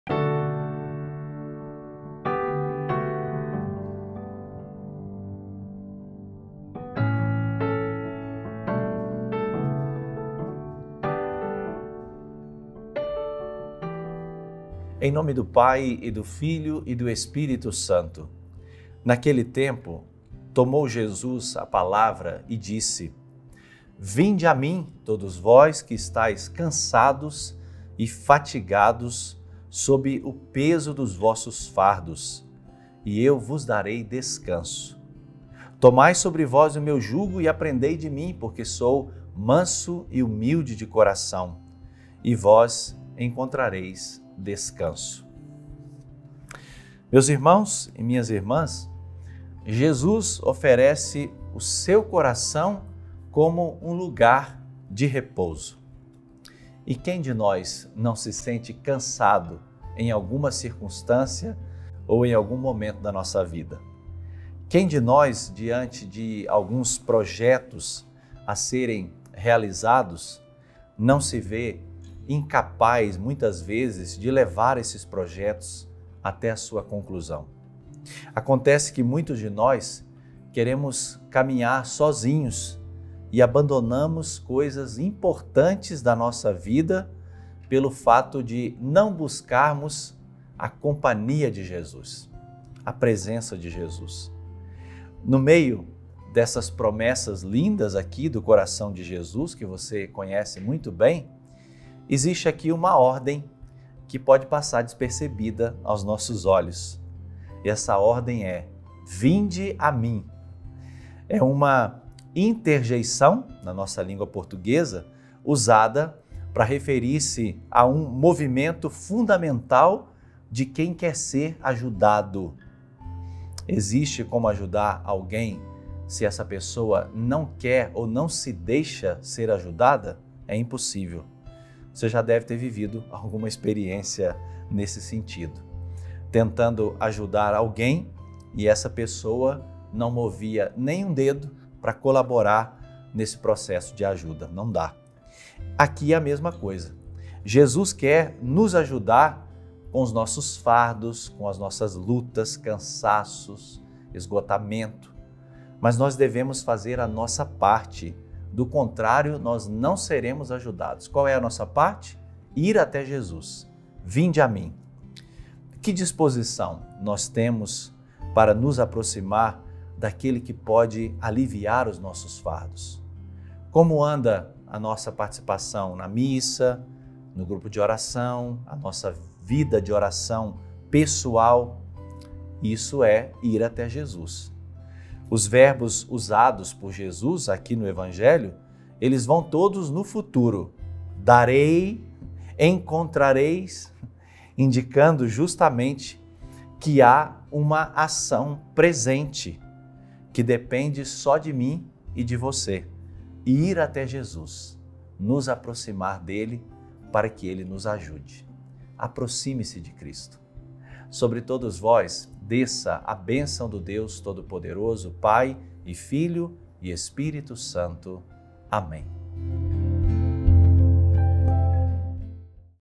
Homilia Diária | Encontrai descanso em Jesus: vinde a mim, todos os cansados | 18 de Julho de 2024
Classificação Indicativa: Livre para todos os públicos

“Naquele tempo, tomou Jesus a palavra e disse: “Vinde a mim, todos vós que estais cansados e fatigados sob o peso dos vossos fardos, e eu vos darei descanso. Tomai sobre vós o meu jugo e aprendei de mim, porque sou manso e humilde de coração, e vós encontrareis descanso.” (Mt 11,28-30)

Meus irmãos e minhas irmãs, Jesus oferece o seu coração como um lugar de repouso. E quem de nós não se sente cansado em alguma circunstância ou em algum momento da nossa vida? Quem de nós, diante de alguns projetos a serem realizados, não se vê incapaz, muitas vezes, de levar esses projetos até a sua conclusão? Acontece que muitos de nós querem caminhar sozinhos e abandonam coisas importantes da vida pelo fato de não buscar a companhia e a presença de Jesus.

No meio dessas promessas lindas do coração de Jesus, que você conhece muito bem, existe uma ordem que pode passar despercebida aos nossos olhos. Essa ordem é: “Vinde a mim”. É uma interjeição na nossa língua portuguesa usada para referir-se a um movimento fundamental de quem quer ser ajudado. Existe como ajudar alguém se essa pessoa não quer ou não se deixa ser ajudada? É impossível! Você já deve ter vivido alguma experiência nesse sentido. Tentando ajudar alguém e a pessoa não movia nem um dedo para colaborar nesse processo de ajuda. Não dá!

Gerente operacional: André Rosa
Direção e produção: Ester Vieira
Câmeras: Jorge Aparecido e Sávio Augusto
Edição e Finalização: Lucas Santana
Tradução: Thaís Mariotto